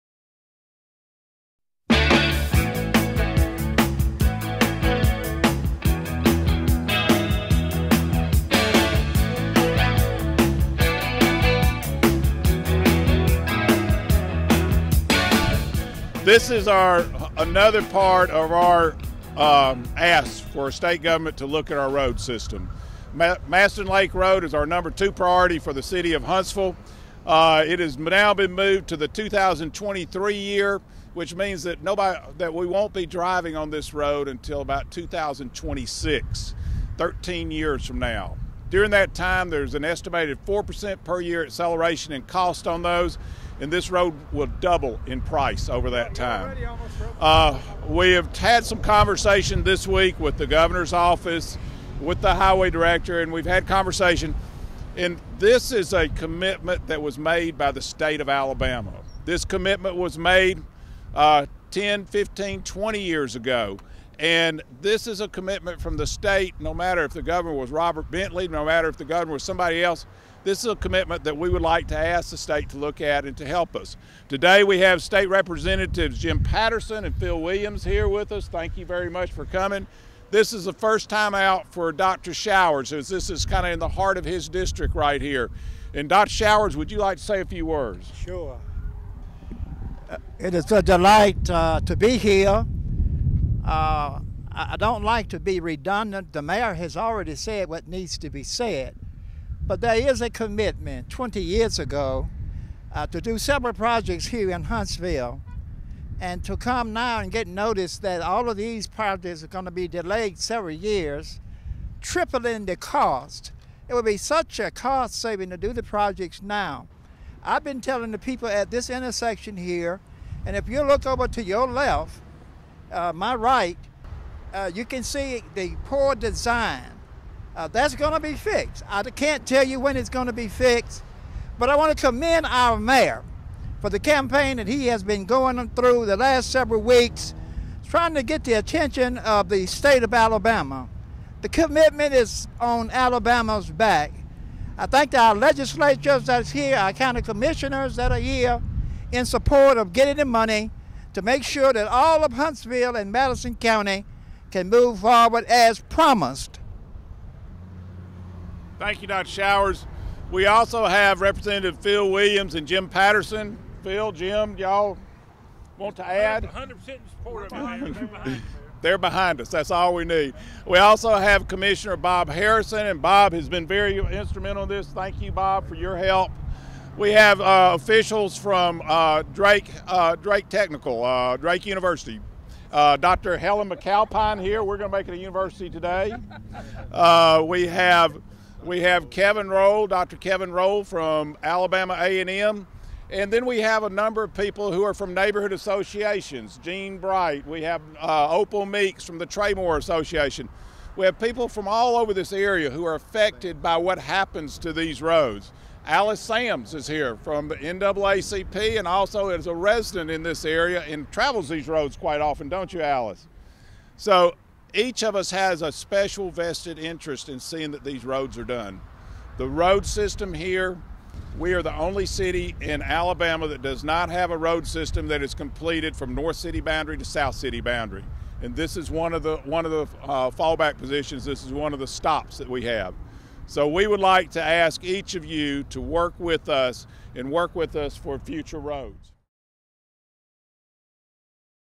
Restore Our Roads: Mastin Lake Road Overpass on North Memorial Parkway
(Huntsville, AL) -The Mastin Lake Road Overpass on North Memorial Parkway is one of 14 casualties in the Alabama Department of Transportation's road budget for Huntsville.

The project has been under design for about 13 years, and ALDOT has decided to further delay the $20 million overpass from its projected start date in 2018 to 2023. The deferral increases the cost of the project to nearly $50 million, and it halts the overall road plan for North Huntsville and future projects for North Memorial Parkway, including the Northern Bypass.

"Huntsville has waited decades for the completion of the Memorial Parkway, north-south corridor, and we cannot withstand any more delays," said Mayor Tommy Battle. "We are rapidly reached the break point where the Parkway will be at capacity before it is ever finished."

ALDOT announced it will defer more than $400 million in critical road projects for Huntsville from five to 10 years due to shortfalls in gas tax revenues. The delay means the roads would not be complete for at least another 10 to 20 years or more.

Madison County ranks 3rd in gas tax revenues generated for the State of Alabama, and the County has sent approximately $72 million per year, on average, from 2000 to 2012 in gas tax receipts to the State.

"We have prioritized our resources and spent millions of taxpayer dollars to support ALDOT in the effort to complete the overpasses on Memorial Parkway," said Shane Davis, Director of Urban Development. "We have done our part in meeting our obligations, and we need the State to uphold their commitment."

Mayor Battle has noted that Huntsville is the only major city in Alabama without one complete limited access highway within its city limits. Montgomery, Birmingham and Mobile benefit from multiple Interstates running through their corridors including I-65, I-85, I-20 and I-10. Yet, Huntsville is listed as the fastest growing metro area in the State of Alabama and #23 in the region over the next five years by the Metro Magnets Index.

Pushing critical road projects further out will only stifle growth and add further congestion.